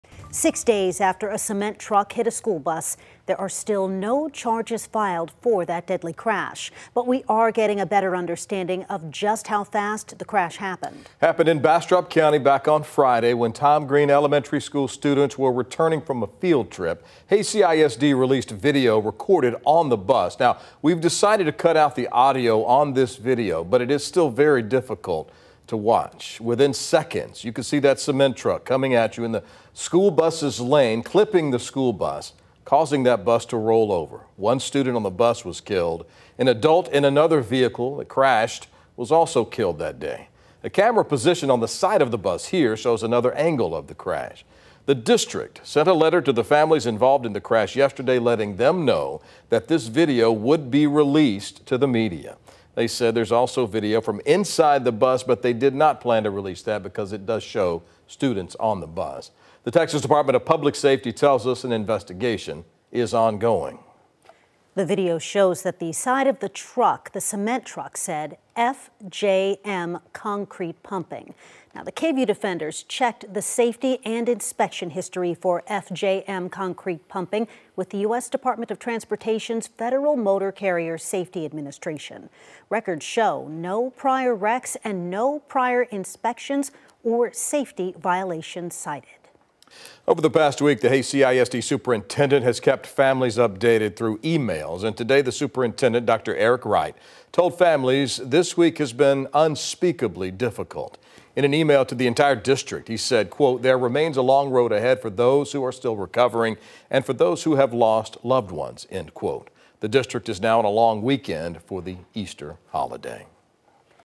Hays CISD releases video of fatal bus crash in Bastrop County
Six days after a cement truck hit a Hays CISD school bus, there are still no charges filed. But we're getting a better understanding of just how fast that crash happened.